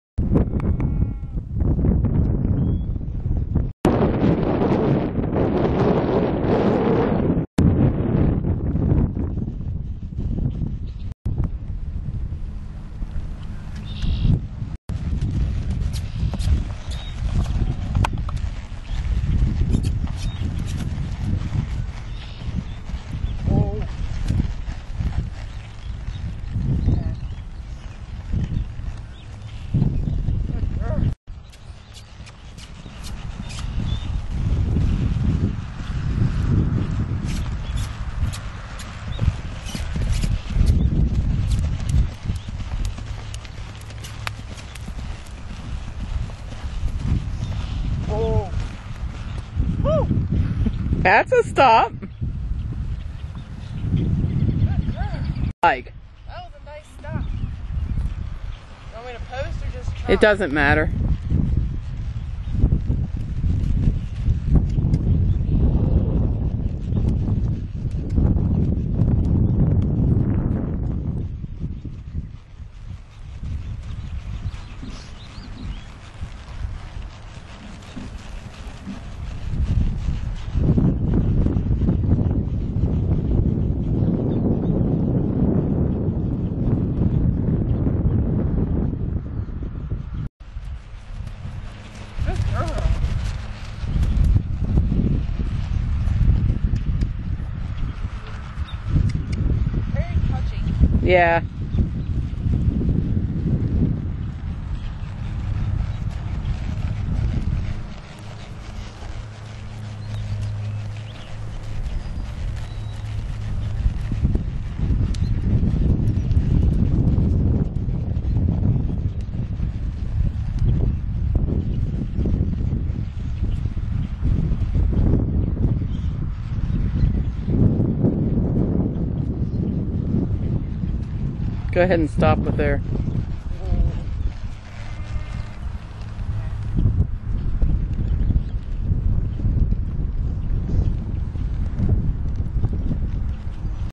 Snazzy ride2
Snazzy Sugar Wagon, owned by Roaring Ridge Paints and Pintos of Wardensville,WV, on her second solo ride on 4/13/13.